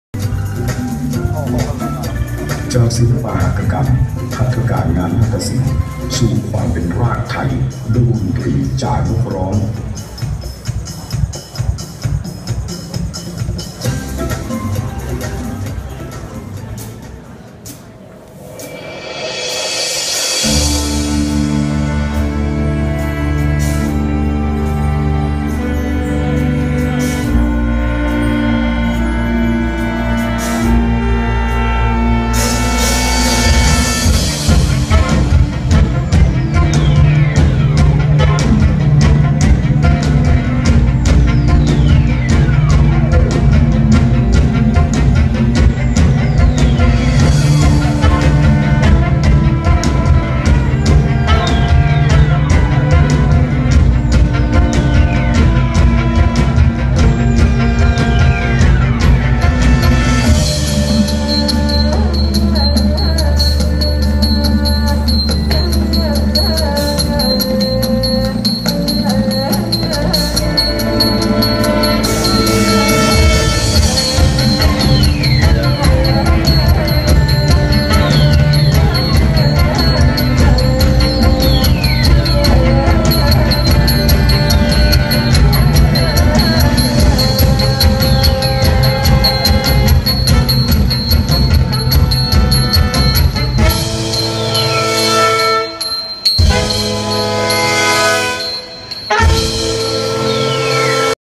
016+sillapa65+การแสดงชุด “รากไทย” โดยนักเรียนโรงเรียนจ่านกร้อง จังหวัดพิษณุโลก
การแสดงชุด “รากไทย” โดยนักเรียนโรงเรียนจ่านกร้อง จังหวัดพิษณุโลก
พิธีเปิดงานศิลปหัตถกรรมนักเรียนระดับชาติ ครั้งที่ ๖๕ ปีการศึกษา ๒๕๕๘
ณ หอประชุมสมภาพ คมสัน (ชั้น ๔ อาคารกาญจนาภิเษก)
โรงเรียนสตรีวิทยา ๒ ในพระราชูปถัมภ์สมเด็จพระศรีนครินทราบรมราชชนนี จังหวัดกรุงเทพมหานคร
วันศุกร์ที่ ๒๙ มกราคม พ.ศ. ๒๕๕๙+video prachoom rangkasikorn